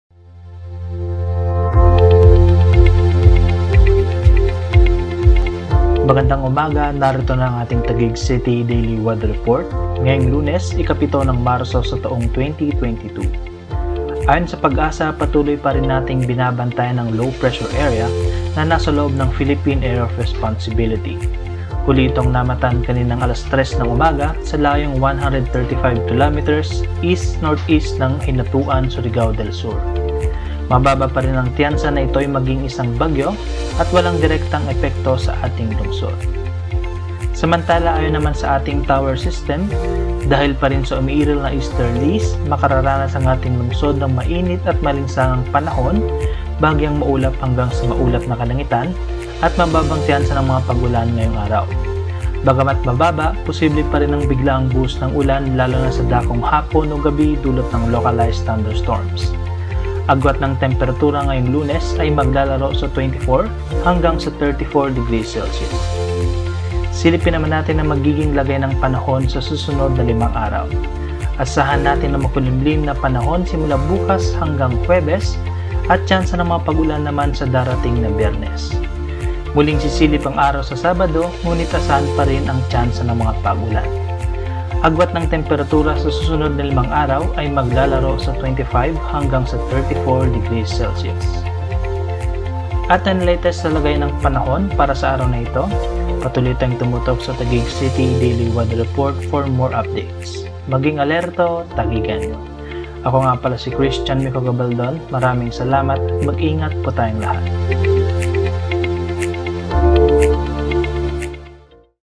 Taguig City Daily Weather Report - March 7, 2022
Alamin ang lagay ng panahon ngayong Lunes at sa susunod na limang araw.